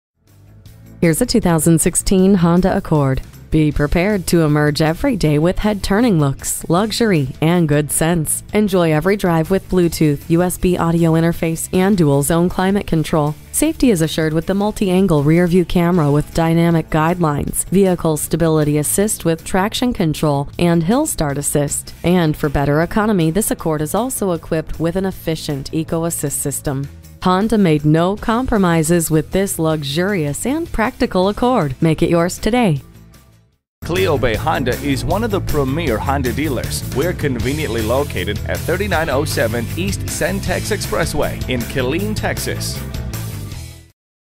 2016 Honda Accord Killeen TX Austin, TX
Year: 2016
Make: Honda
Model: Accord
Trim: LX w/Honda Sensing
Engine: 2.4L 4 cyls
Transmission: CVT (Continuously Variable)
Color: White
Interior: Ivory
Mileage: 5815
Stock #: RL5022
VIN: 1HGCR2F44GA008598
New Arrival! CarFax 1-Owner LOW MILES This 2016 Honda Accord Sedan LX w/Honda Sensing will sell fast -Bluetooth ABS Brakes Based on the excellent condition of this vehicle along with the options and color this Honda Accord Sedan is sure to sell fast. -Front Wheel Drive -CARFAX 1-Owner and many other amenities that are sure to please.

Address:
3907 E Cen-Tex Expy